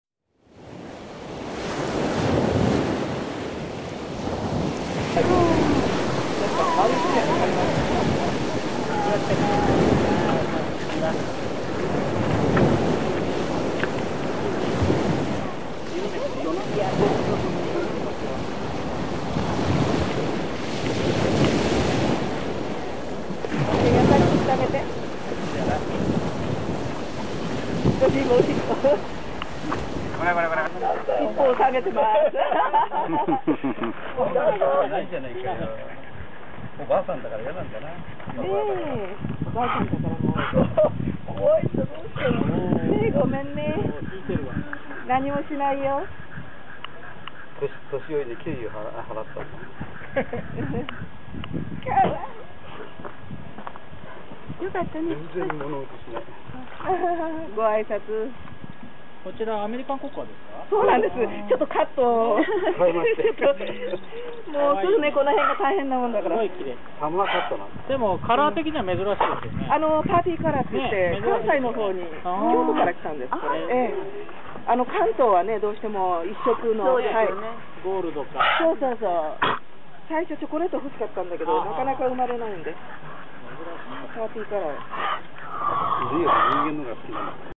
近所にしずかのお友達 Staffordshire Bull Terrier
まさか近所にStaffordshire Bull Terrierがいたなんて！1歳半のメス「こころ」ちゃん。ブリンドルカラーがきれいです。